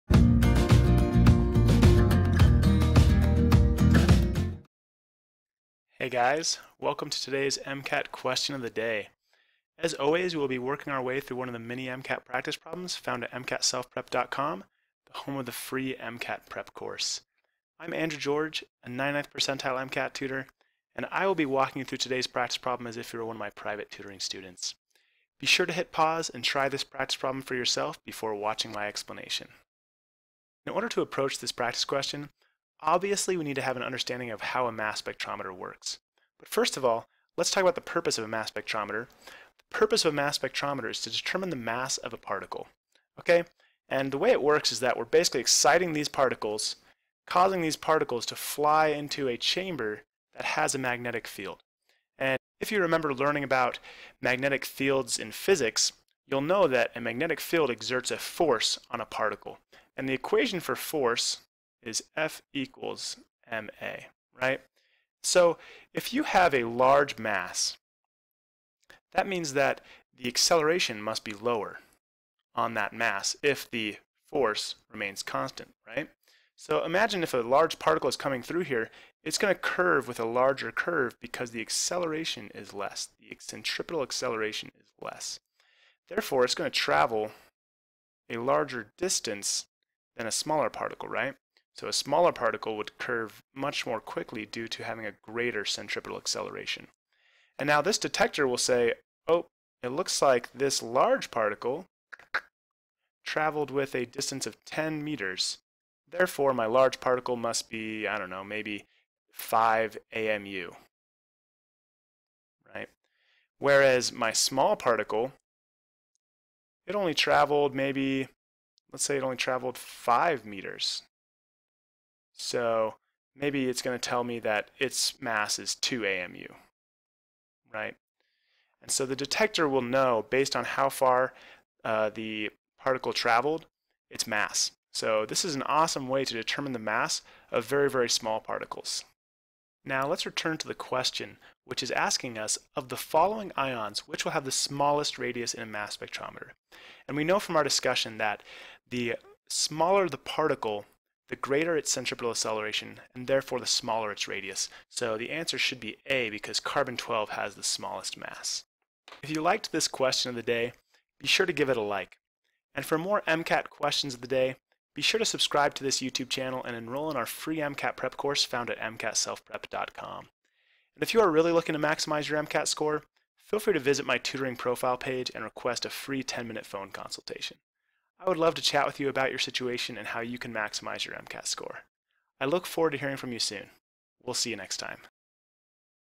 MCAT Question of the Day: Mass Spectrometer and Mass Spectrometry
In this MCAT Question of the Day, we will be taking a look the inner workings of a mass spectrometer. For more MCAT tips and Questions of the Day: